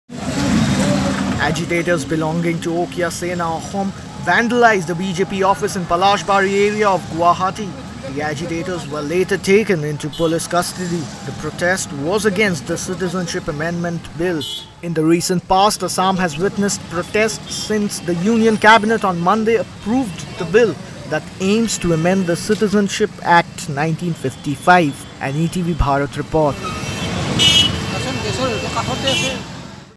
BJP office vandalised in Assam
Guwahati: As protests against the Citizenship (Amendment) Bill 2016 continued in Assam, a group of agitators vandalised a BJP office near here, police said on Friday.